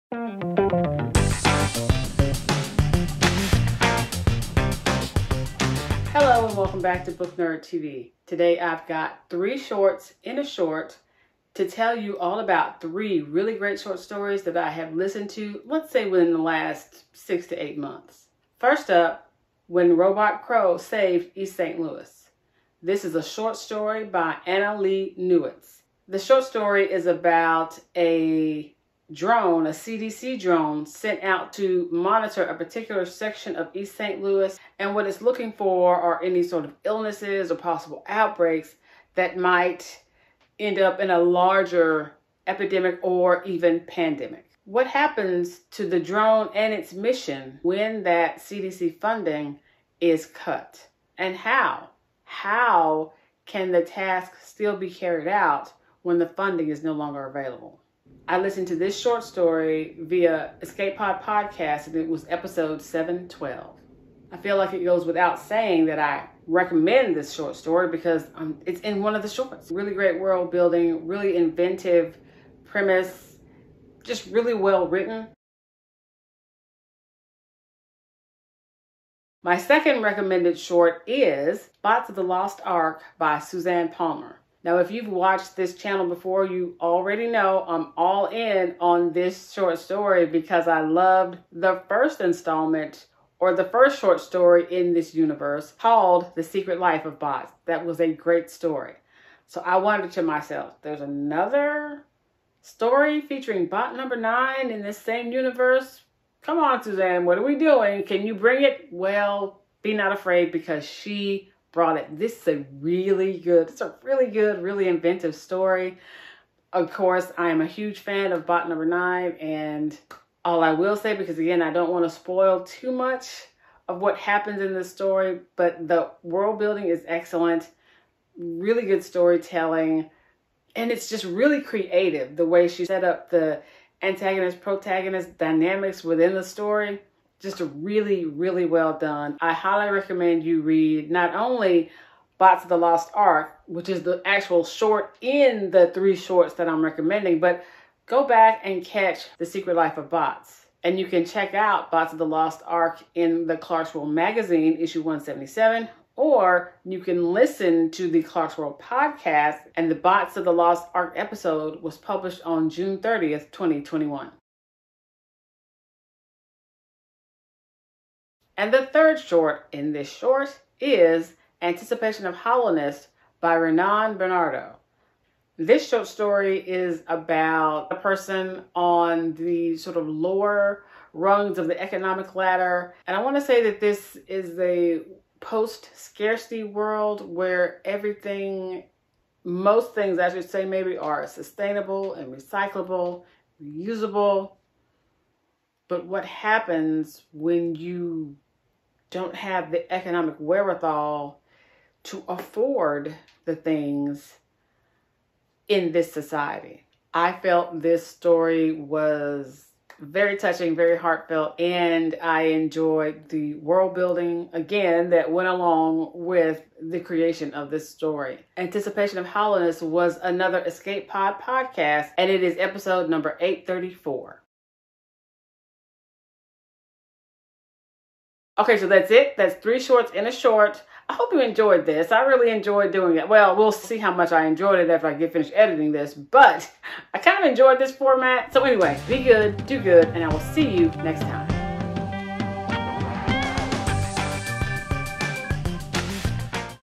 3 Shorts in a Short(ish)
I recommend 3 science fiction based, bot-related short stories for you to enjoy via podcast or magazine format.

Short Stories recommended:
When Robot Crow Saved East St. Louis by Annalee Newitz
Bots of the Lost Ark by Suzanne Palmer
Anticipation of Hollowness by Renan Bernardo

Short Stories mentioned:
The Secret Life of Bots By Suzanne Palmer

Podcasts mentioned:
EscapePod
Clarkesworld Podcast